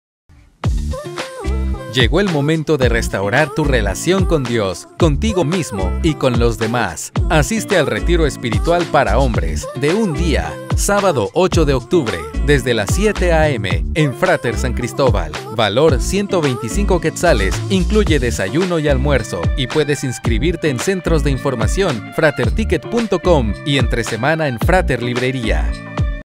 Retiro Espiritual para hombres 💪🏼
Restaura tu relación con Dios, contigo mismo y con los demás. Asiste al retiro espiritual para hombres de 1 día.

⌚️7 am
📍frater sancristobal
🎟Q125